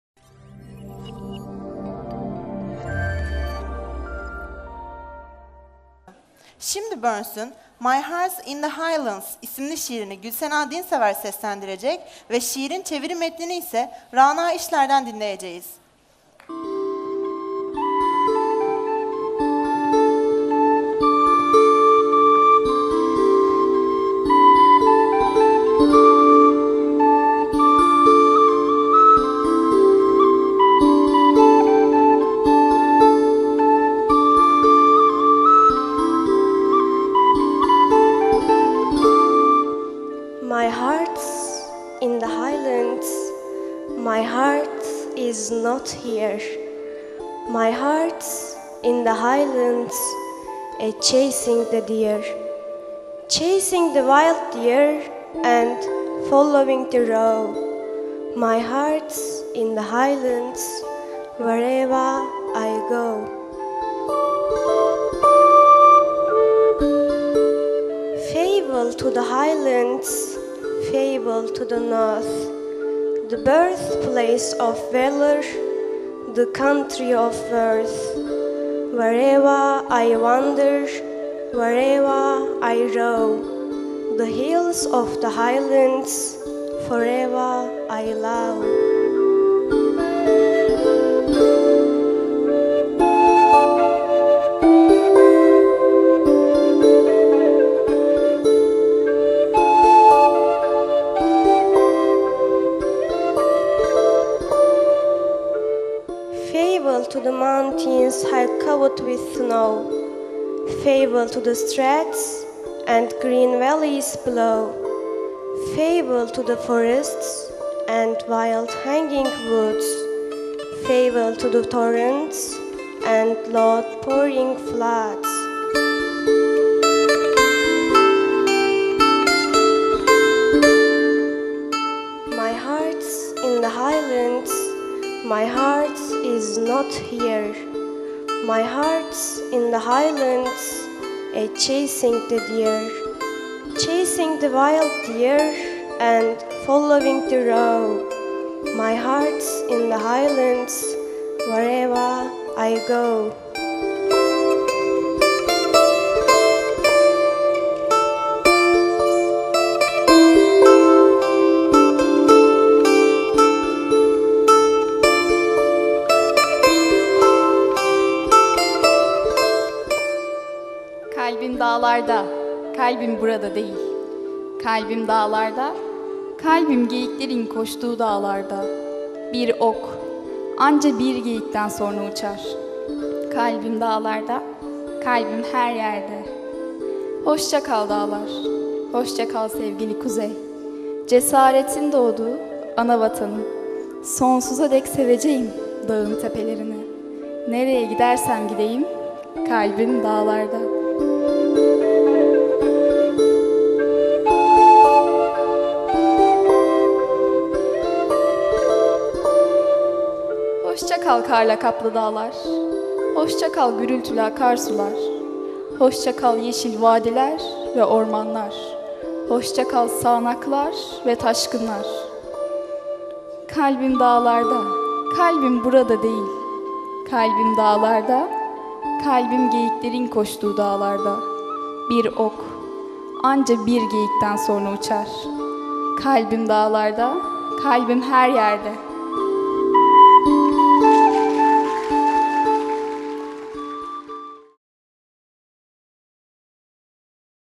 18. GÖNÜL HARMANI ŞİİR AKŞAMI
Yorum: Gülsena DİNSEVER
Çeviri Yorum: Rana İŞLER
Robert Burns, My heart is in the Highlands